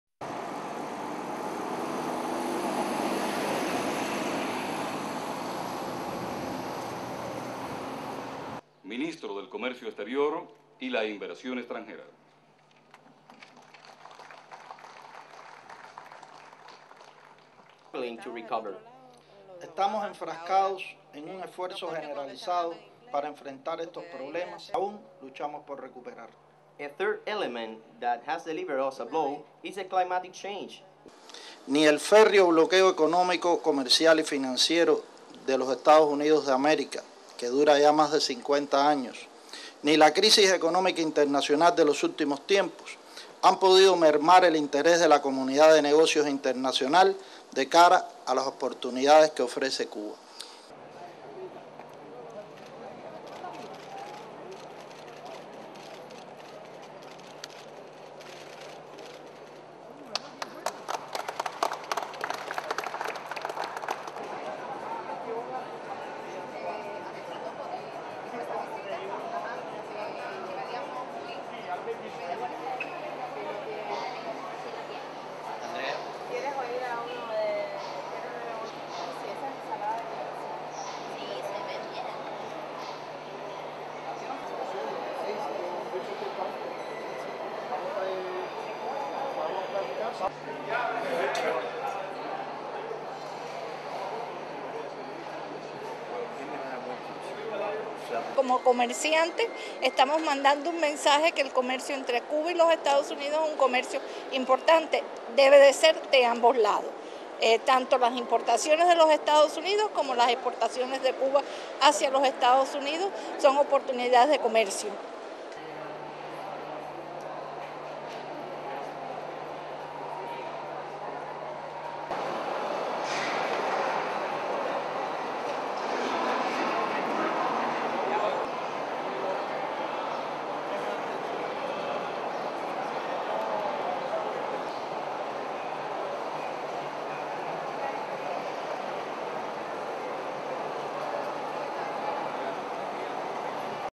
Cuba hosts the 28th International Trade Fair in Havana
1. Wide of exterior of Havana International Trade Fair
2. Wide of opening ceremony
3. Mid of Cuban authorities and Rodrigo Malmierca, Cuban Minister for Foreign Trade at podium
4. Mid of officials (left to right) Jose Ramon Fernandez, Vice President of Cuban Council of Ministers, Ricardo Cabrisas, Vice President of Cuban Council of Ministers, Ulises Rosales, former Cuban Agriculture Minister and Fidel Castro Diaz Balart, eldest son of Fidel Castro, in background
5. SOUNDBITE (Spanish) Rodrigo Malmierca, Cuban Minister of Foreign Trade:
"Neither the strong economic, commercial and financial Blockade of the United States of America, that has lasted over 50 years, neither the international economic crisis of recent times have been able to decrease the interest of the international business community to the opportunities offered by Cuba."
6. Push in on Cabrisas, Fernandez and Rosales cutting ribbon
7. Push in on Malmierca talking to Brazilian authorities
8. Tilt down of North American products on stand
9. Wide of US stand
10. Mid of US products
11. Push in on trader with US and Cuban flags on his t-shirt
12. SOUNDBITE (Spanish) Maria Conchita Mendez, Trade Development and Sales Director of Alabama Port:
"As traders we are sending a message that trade between Cuba and the United States is very important and should be from both sides. The US imports and the exports from Cuba to the United States are trading opportunities."
13. Wide of US Stand and sign in background reading (English) "Ocean Shipping Services. USA/CUBA"
14. Tilt down "Habanos S.A." Stand
15. Wide of "Havana Club" Stand
16. Pull out of Cuban stands
STORYLINE
Cuba hosted the 28th International Fair in Havana on Monday with the country's Minister for Foreign Trade reporting improved export figures.
Rodrigo Malmierca said that the country's exports had risen 21 percent in the first nine months of the year, while imports were up only one percent.
"Neither the strong economic, commercial and financial Blockade of the United States of America, that has lasted over 50 years, nor the international economic crisis of recent times have been able to decrease the interest of the international business community to the opportunities offered by Cuba," Malmierca said.
Malmierca said that more than 300 foreign companies were investing in Cuba.
No Cuban officials were available to say how many US companies had signed up to participate in the trade fair, but a stand with US products was set up at the fair.
"As traders we are sending a message that trade between Cuba and the United States is very important and should be from both sides. The US imports and the exports from Cuba to the United States are trading opportunities,"said Maria Conchita Mendez, Trade Development and Sales Director of Alabama Port.
In 2009, official figures showed that US purchases totalled 729 (m) million US dollars, down 30 percentage from the previous year.